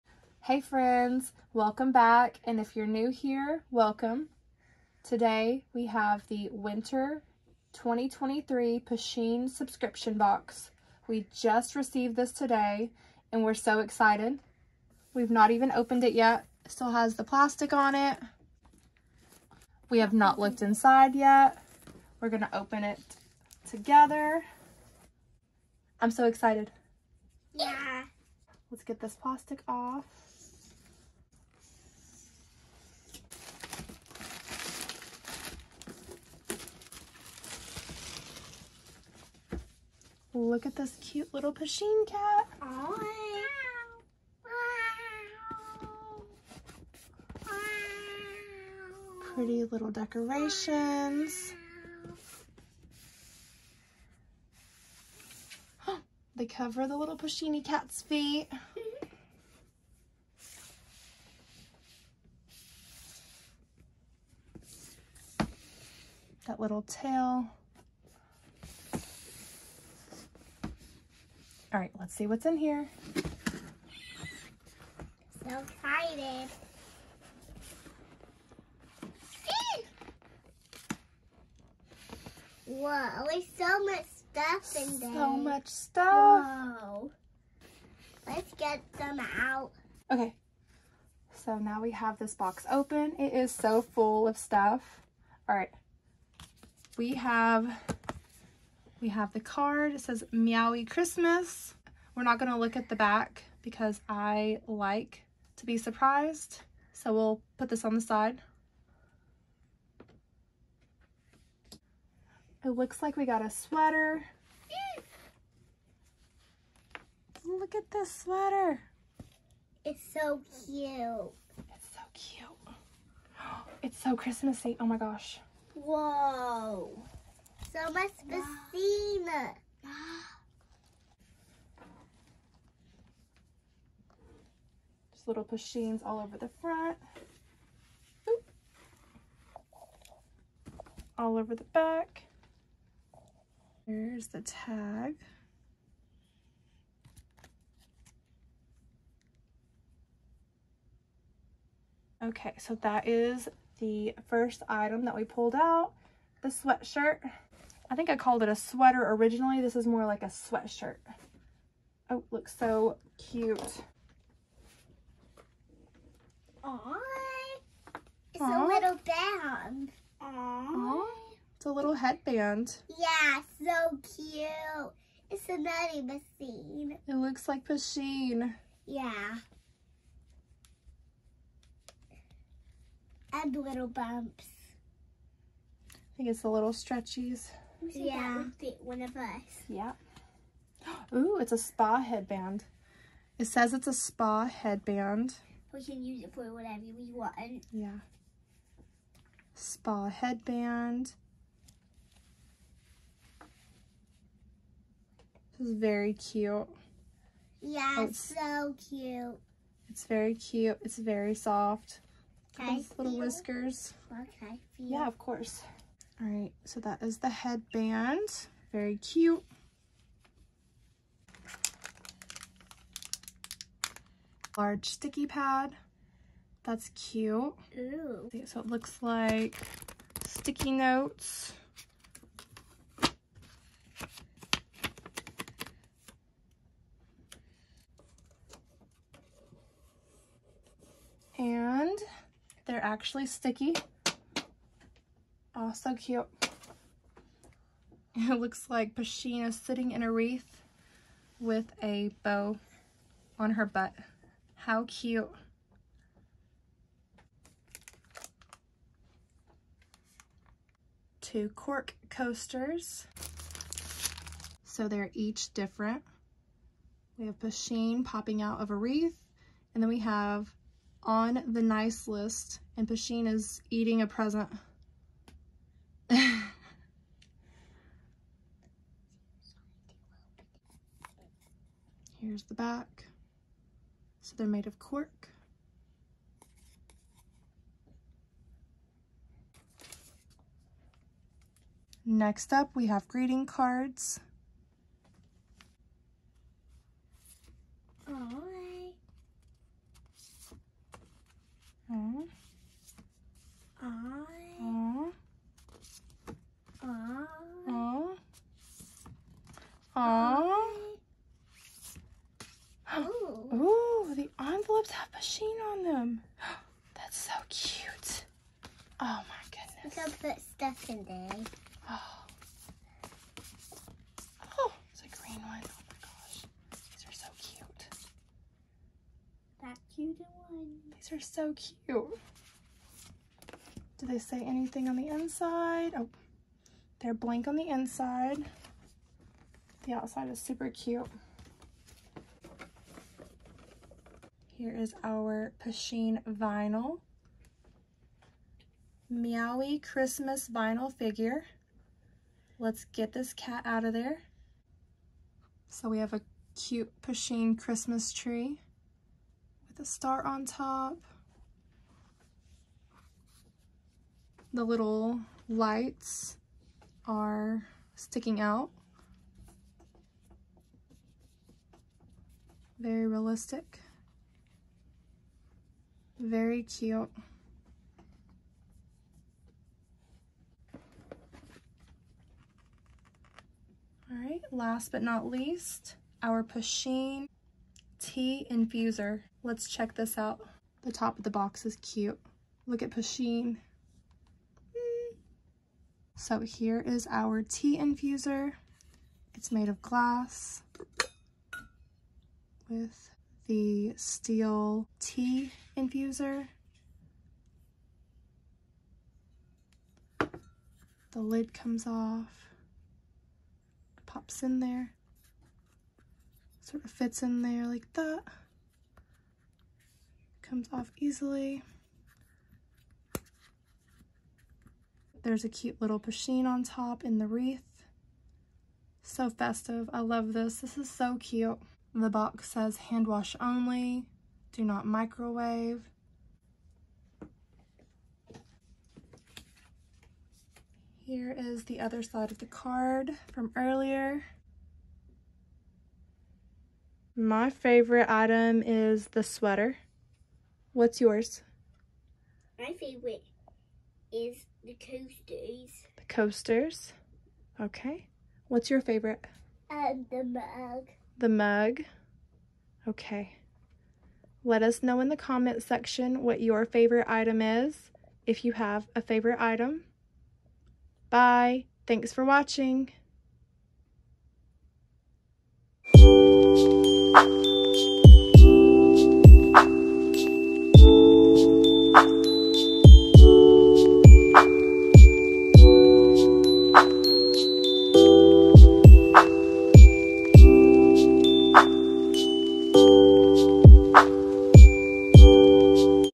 🎄💫 Pusheen Winter 2023 Subscription Box ~ Christmas Pusheen ~ Holiday Pusheen Cat
*This video is made for adult and young adults*

0:00 - Intro
1:16 - Opening box
1:52 - 1st item
2:44 - 2nd item
3:41 - 3rd item
4:18 - 4th item
4:50 - 5th item
5:43 - 6th item
6:21 - 7th item

We are opening our winter Pusheen box in this video!